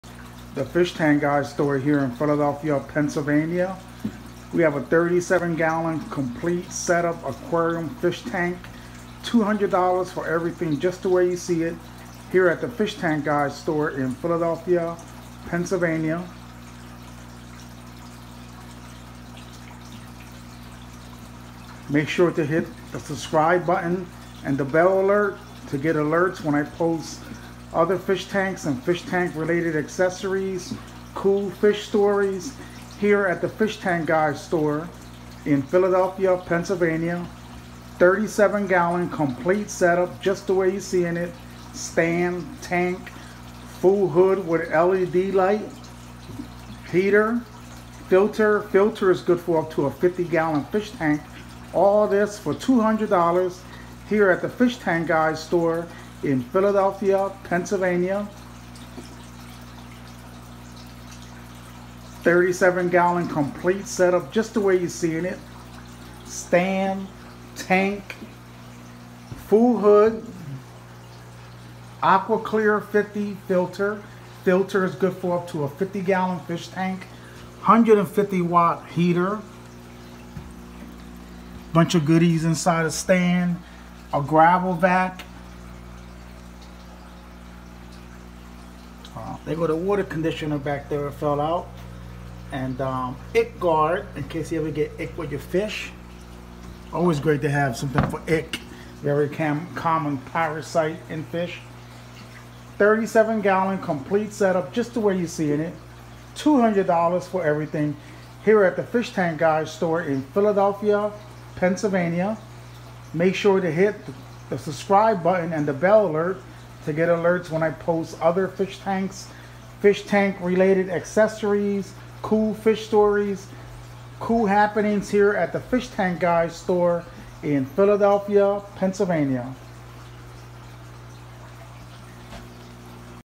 37 gallon aquarium fish tank complete set up $200
37 gallon Aquarium fish tank complete set up $200
unusual size tank, taller than a common 29 gallon tank
you're getting the tank, stand, full top with bright white LED light, aqua clear filter (good for up to a 50 gallon fish tank ) heater, water conditioner, gravel vac and ick guard

*** holds water fine, see pics

for more info checkout my YouTube channel youtube/thefishtankguystore

tank measurements approximately 30" Long x 23" High x 12" Wide
stand measurements approximately 31" Long x 28" High x 13" Wide

*** can deliver locally for a fee, Roadie.com can deliver nationally contact them for rates

price is firm, unless you don't need everything that is included, then I can discount from price

for your convenience we accept all major CREDIT CARDS

FOR MORE INFO The Fish Tank Guy two15 nine17 eight730